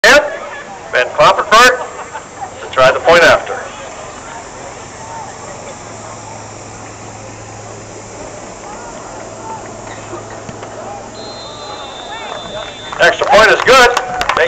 08/27/2011 - Mankato Loyola Crusaders @ MACCRAY Wolverines Vars Football Game (Inside the 4th Qtr)
Following the Lee Segura 4-Yard TD Run, MACCRAY's DJ VonKlompenburg kicked the Extra Point.